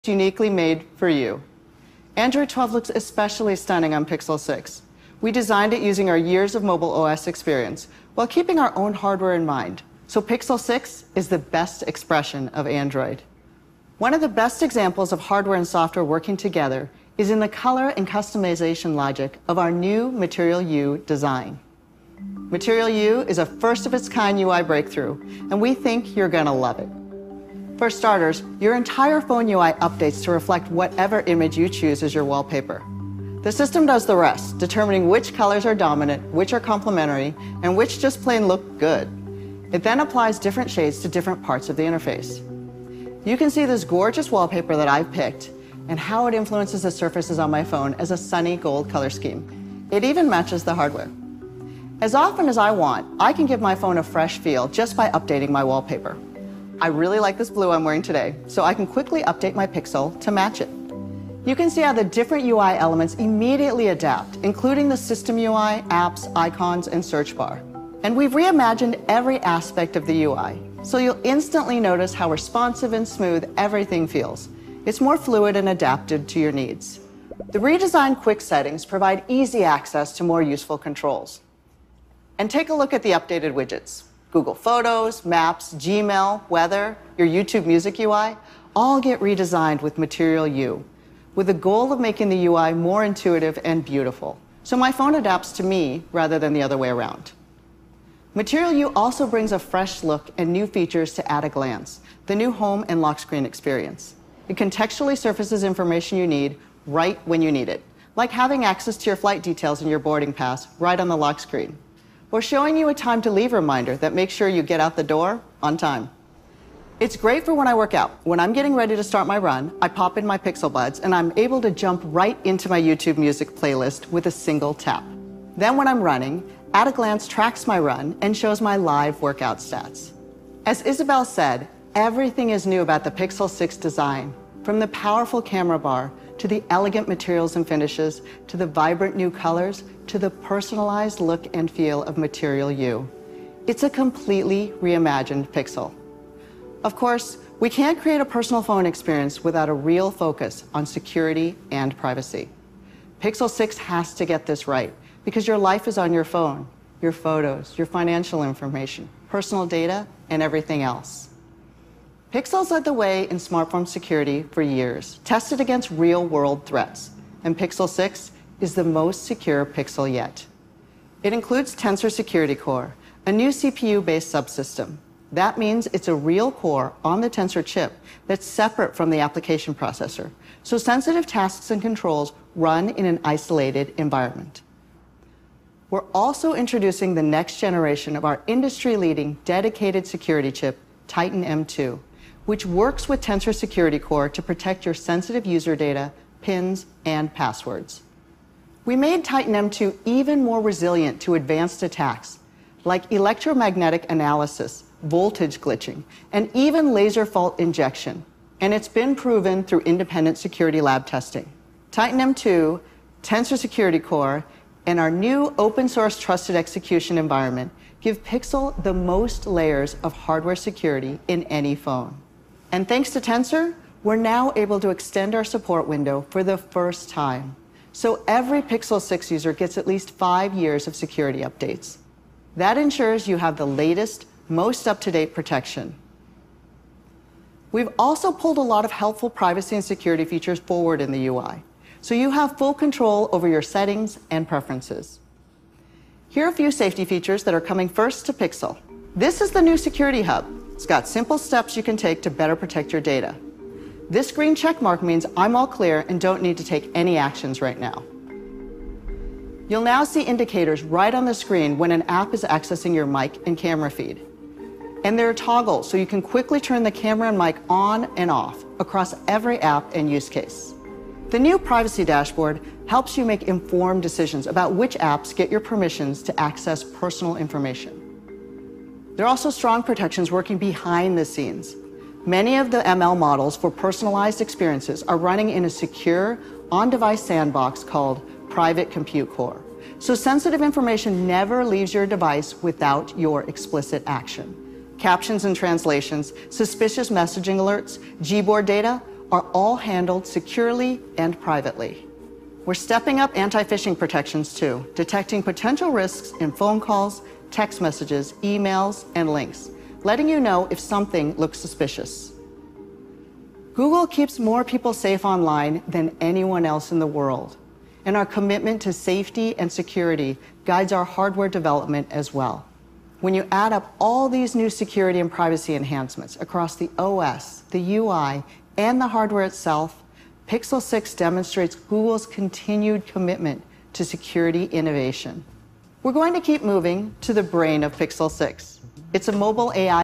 Android 12 on Pixel 6 adapts to your personal style
Google gives us the first look at Android 12 on its new Pixel 6 phones with updated widgets and customization options.